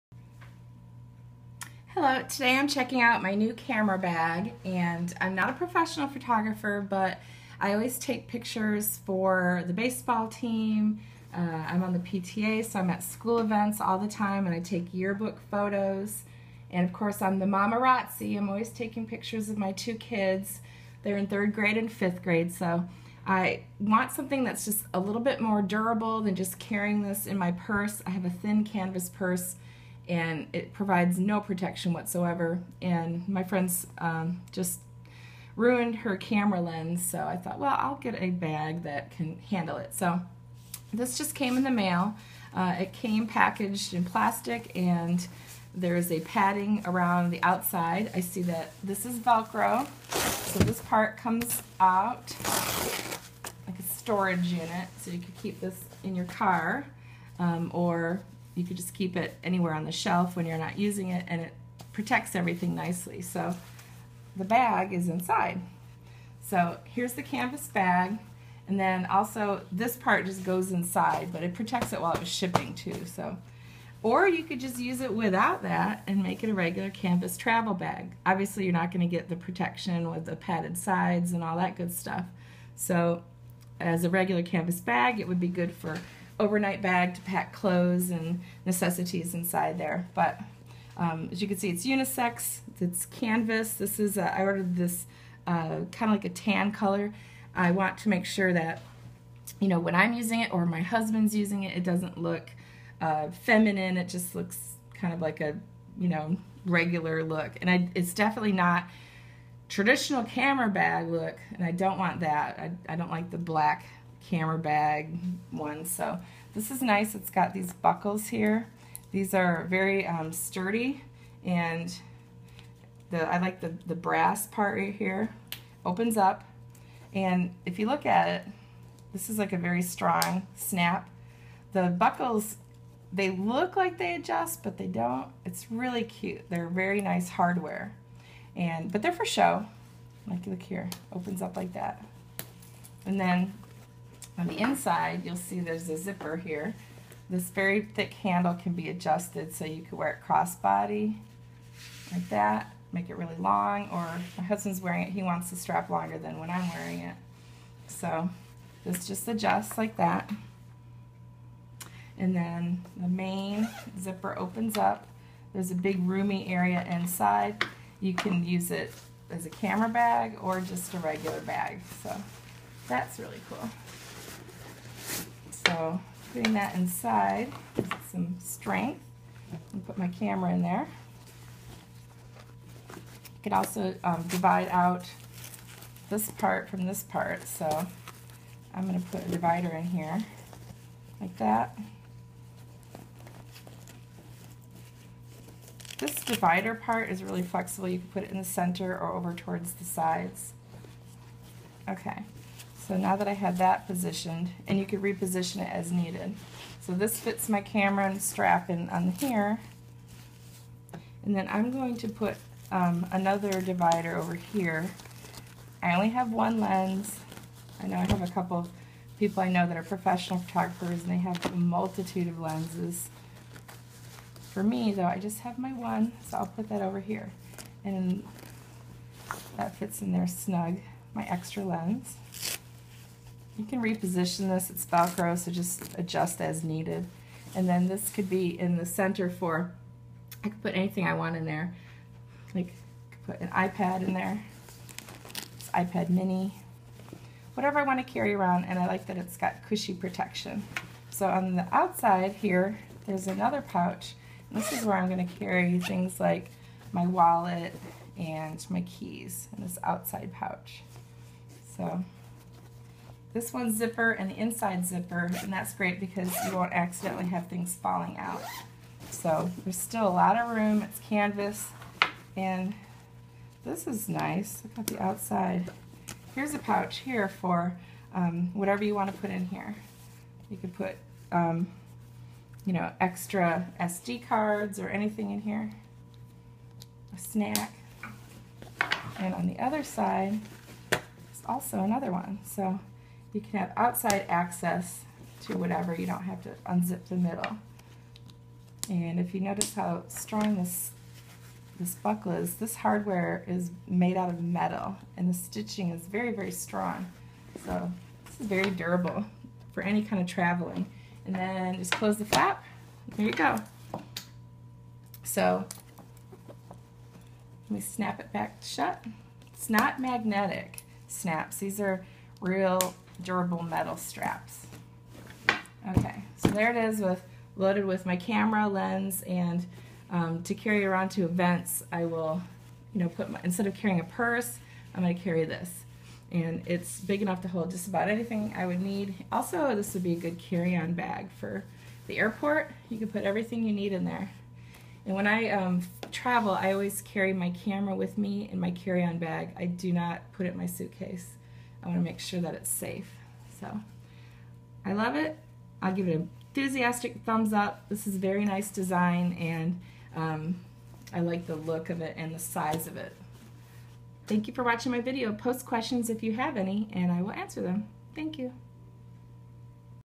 SLR Camera Bag with Removable Shockproof Insert! Canvas messenger bag style!
I adore this camera bag! It has plenty of room to store my camera, additional lens, iPad mini, phone, power bank and cables, keys, wallet, plus LOTS more room for whatever I need to stuff in there! This would be ideal for a carry-on bag for travel as well. I like to bring my camera in my carry-on bag to keep it safe on the airplane.

I always take pics at games & school events! My two boys call me "The Mamarazzi" since I'm always taking pics! I grew up with a hippie mom who never had a camera, so I only have a smal handful of photos from my entire childhood (that my Grandma took during rare visits!). I suppose I'm overcompensating now! LOL

I had an ugly black standard looking camera case, but I never like to carry that around AND my purse. I have always stuffed my Nikon in my purse and never have room for my bigger "zoom lens" (yeah, I know, I'm not a pro as you can tell. I forgot what the additional lens size is). I like the zoom lens for sporting events! So far, I'm really pleased with the look of this bag, and how big it is!

Pros:
1) Rugged and durable looking, with a cool, vintage look. I like the unisex design, so my husband can carry it around too.
2) This will ensure that I carry my Nikon camera around more often. It's hard to capture decent images on my phone!
3) Inside padded compartment has velcro dividers that can be removed or moved around to adjust sizing of sections. Very versatile!
4) Remove the inner compartment if you just want to use it for an overnight bag or roomier carry-on bag to pack clothes, etc.
5) The front snaps on the flap, and the side smaller pocket flap snaps are very durable. They snap tightly.
6) Zipper for front pocket and main pocket is very strong and sturdy, ensuring items don't fall out. Sturdy zipper pulls!
7) Thick 2 inch shoulder strap is comfortable to wear, and easily adjustable to make the strap longer if you want to carry it cross-body style.

Cons:
I'm totally satisfied with this bag! It's so versatile and different from a traditional camera bag!

Overall, this provides the look and style I want, without compromising protection for the camera. I can even fit a mini tripod in there and more equipment as I add on! Love it.

Amazon item name:
SLR Camera Bag, Bukm Waterproof Canvas Messenger DSLR Camera Shoulder Bag with Shockproof Inseert for Digital Cameras, Tablets, Phones, Video Recorder Photography - Khaki Brown
by Bukm